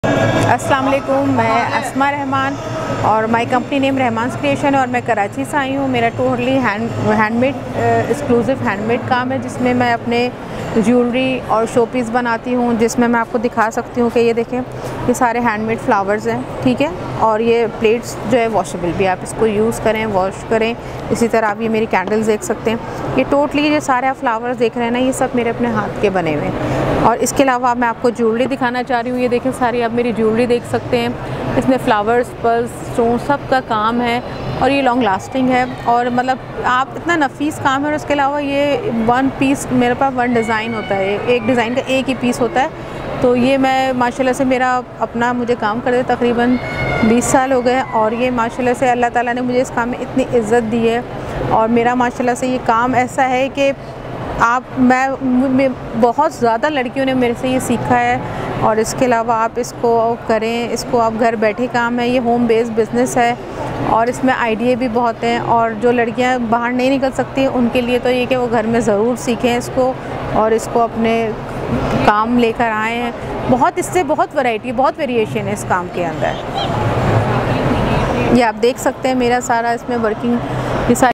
Handmade Jewellery Rehmans Creations Women Expo 10th WEXNET 2022 March 17-18 Expo Centre Lahore
Handmade Jewellery Rehmans Creations Asma Rehman 17 & 18 March 2022
WEXNET (Expo Center Lahore)
Jewellery, Woman Clothes, Handicrafts.
Beauty Products.
Punjab, Sindh, KPK, Blochistan, Pakistan

Order by Email Pakistani Products Women Expo 10th WEXNET 2022 March 17-18 Expo Centre Johar Town Lahore Pakistan.
WEXNET 2022
Preparations for the country's premiere exhibition dedicated solely to the promotion of women entrepreneurs, the WEXNET exhibition, are in full swing at Lahore. The organizing body, the Trade Development Authority of Pakistan is holding the 8th edition of the mega event WEXNET 2022, at Expo Centre Lahore, from 17st to 118th March, 2022. For the first time ever the exhibition will feature international exhibitors apart from an assortment of domestic participants, whereas special regional quotas have been reserved for female business women residing in Baluchistan, Gilgit Baltistan and Khyber Pakhthunkhwa.

The event is being widely publicized throughout the country, along with international promotional efforts through Pakistan's Commercial Sections spread across six continents. Invitations to prominent women entrepreneurs and thinkers across the region, from Thailand, Malaysia, Middle East and Turkey are being sent out, whilst international speakers at the event's opening activity, the "WEXNET Women of Substance Conference", are being hailed from the world over.

WEXNET is a formalized platform which provides women with the opportunity to exhibit their products, network with each other, learn from others' experiences and form collaborations and partnerships for mutual benefits. Organized for the first time in 2001, WEXNET has become a regular feature of the TDAP agenda.

This year, an effort will be made for participation of foreign exhibitors, as well as representation from all regions of the country. The one-day conference will host delegates who will talk about women in the workplace, hurdles they that are commonly faced, while sharing their experiences and success stories. A Fashion show will be held where local fashion schools will be invited to participate for which special prizes will be distributed. The event itself will be a three-day affair, whereby budding female entrepreneurs will display their product lines to international delegates, local dignitaries, corporate visitors and the general public.